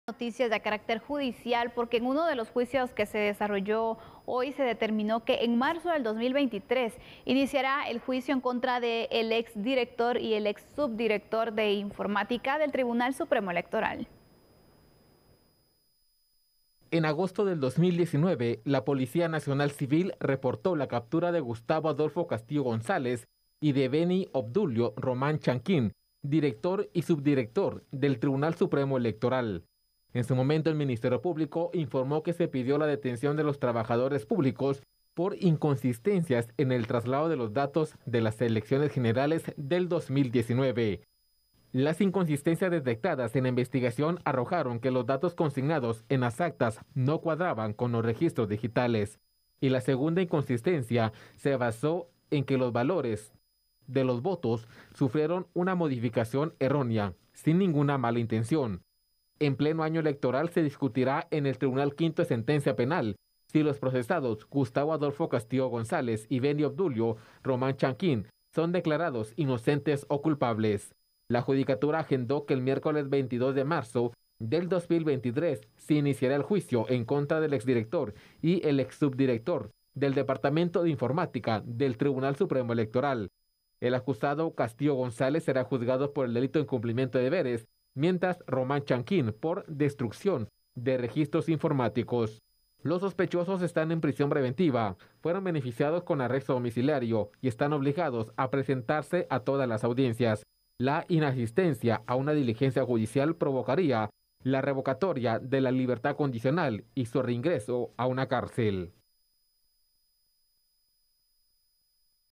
Extrabajadores de informática del TSE enfrentarán juicio
Hoy se determinó que en marzo del próximo año iniciará el juicio contra el exdirector y exsubdirector del Tribunal Supremo Electoral.